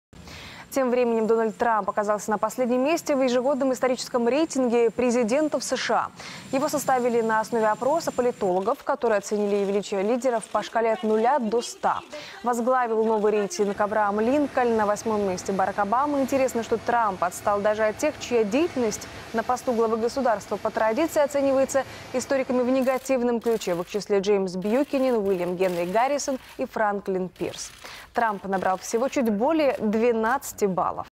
Дональд Трамп оказался на последнем месте в рейтинге президентов США
Рейтинг составили на основе опроса политологов, которые оценили величие лидеров по шкале от 0 до 100. Возглавил новый рейтинг Авраам Линкольн. На восьмом месте Барак Обама. Интересно, что Трамп отстал даже от тех, чья деятельность на посту главы государства по традиции оценивается историками в негативном ключе, в их числе Джеймс Бьюкенен, Уильям Генри Гаррисон и Франклин Пирс. Трамп набрал всего чуть более 12 баллов.